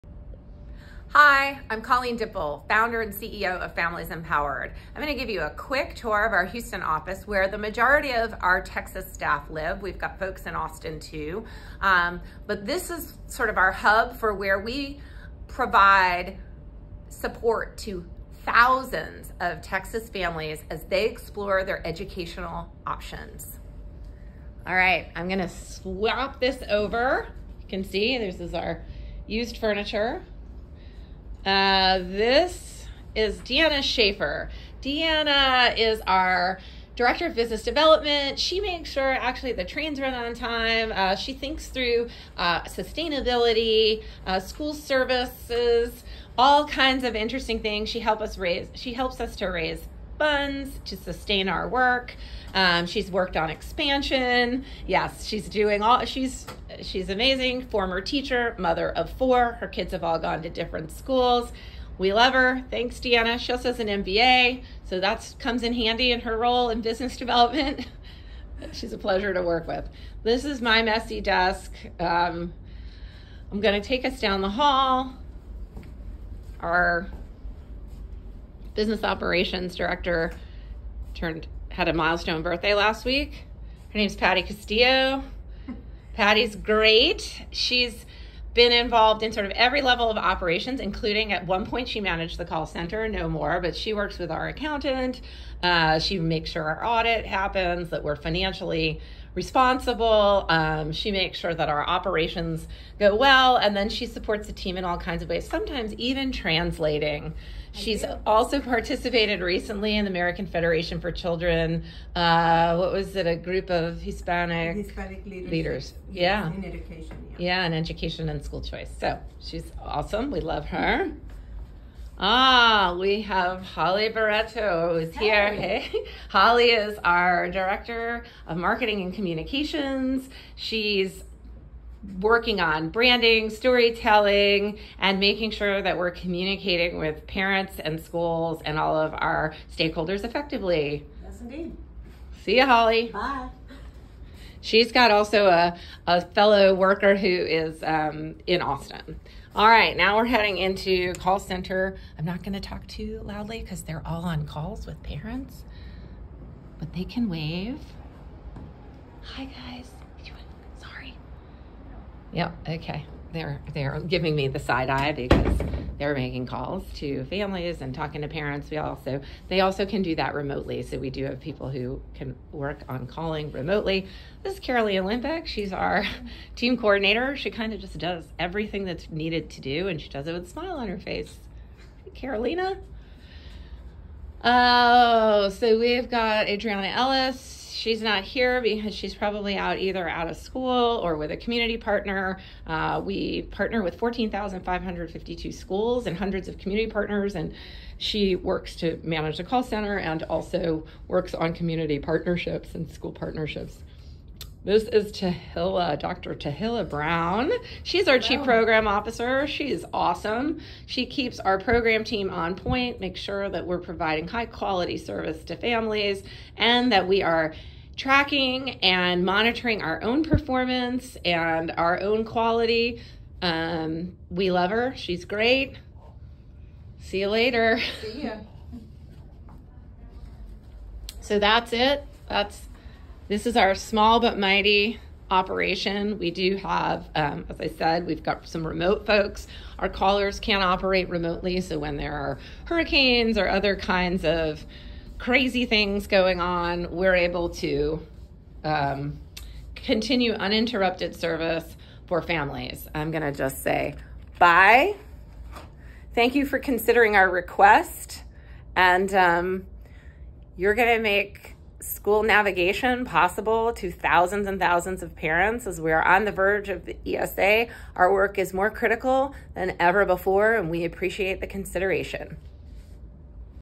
2024 VELA Grant Submission
Hear our Founder and CEO, Colleen Dippel, introduce the Families Empowered team and explore how we've provided personalized, bilingual service to Texas families for the last 15 years.